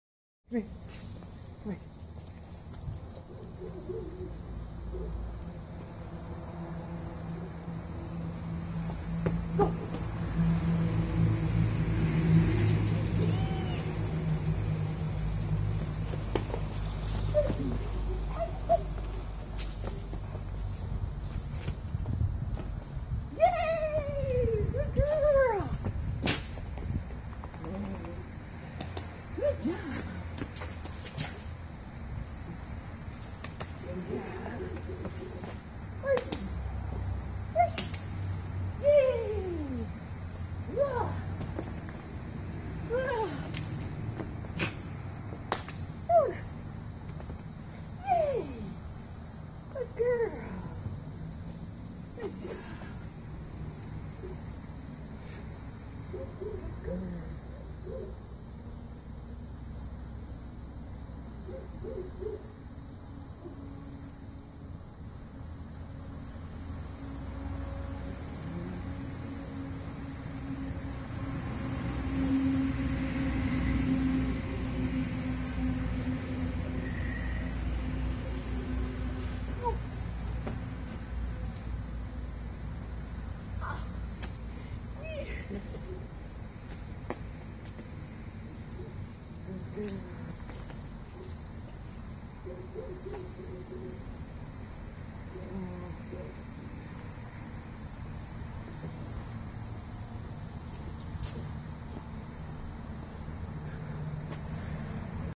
Yuna Personal Play 2
Playing in front of our garage. Second video of personal play for Fenzi
online play class